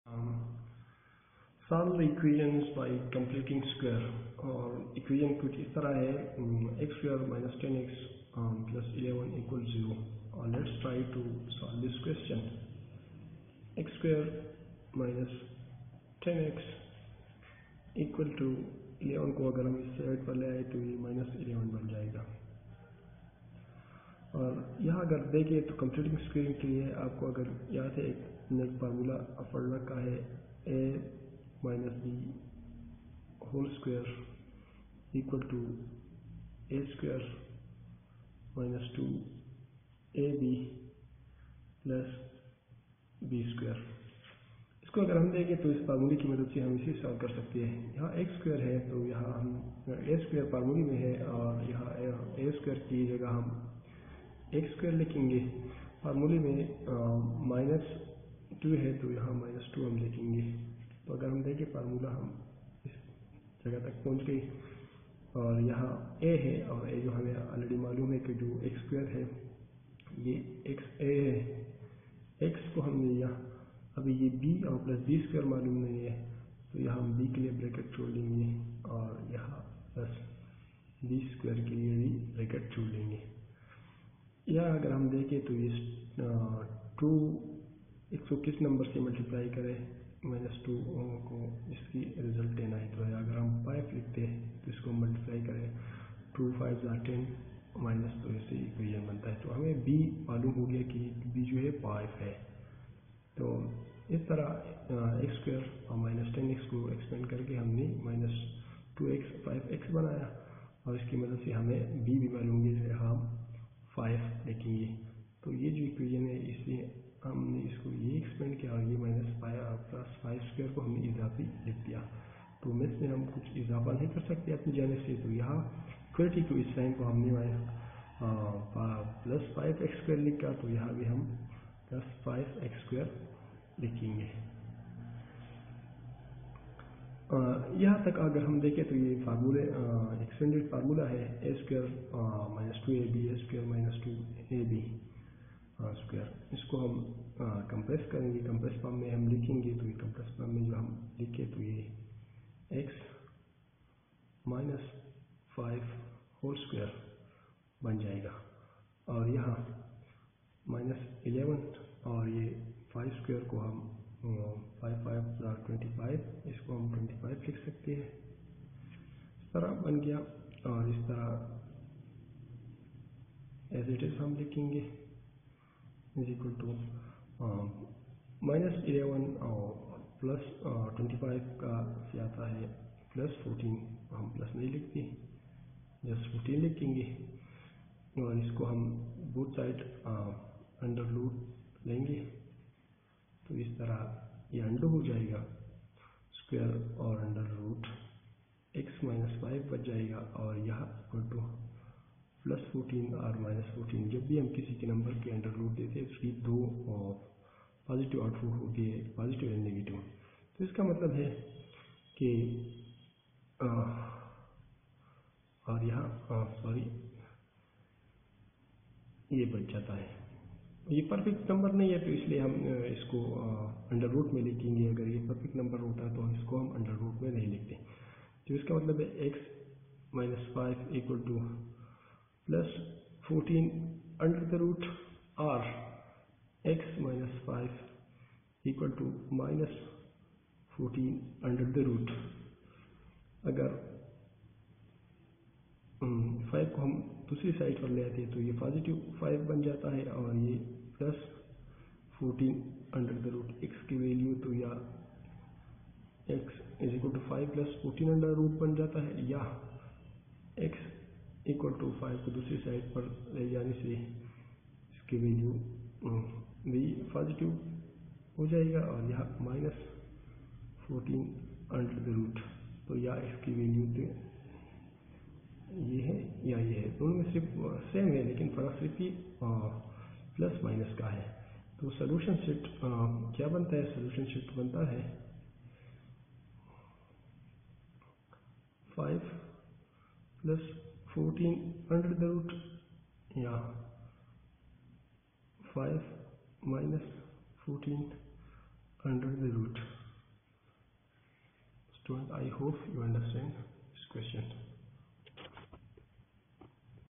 quadratic formula | quadratic equation formula |quadratic equation standard form
Completing the Square is a method used to solve a quadratic equation by changing the form of the equation so that the left side is a perfect square trinomial . To solve ax2+bx+c=0 by completing the square: 1. Transform the equation so that the constant term, c , is alone on the right side